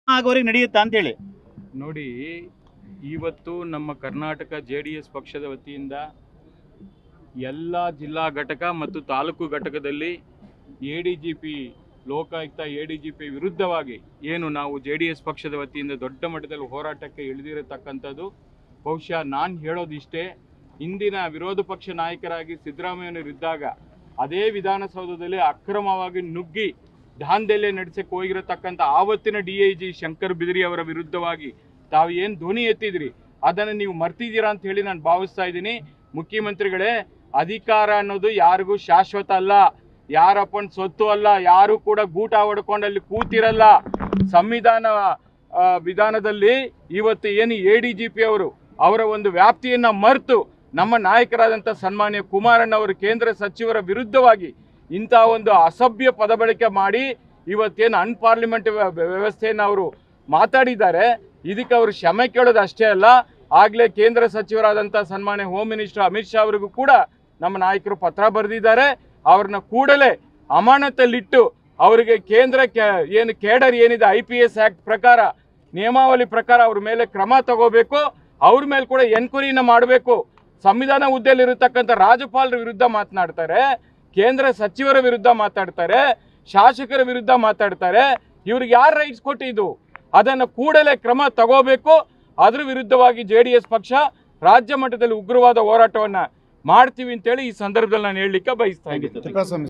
T.a. Sharavana : ಶಾಸಕರ ವಿರುದ್ದ ಮಾತನಾಡ್ತಾರೆ ಇವರಿಗೆ ಯಾರು ರೈಟ್ಸ್ ಕೊಟ್ಟಿದು...? News 1 Karnataka
T.a. Sharavana : ಶಾಸಕರ ವಿರುದ್ದ ಮಾತನಾಡ್ತಾರೆ ಇವರಿಗೆ ಯಾರು ರೈಟ್ಸ್ ಕೊಟ್ಟಿದು...? News 1 Karnataka 24x7

ಡಾ.|| ಸಿ.ಎಸ್.ರಘು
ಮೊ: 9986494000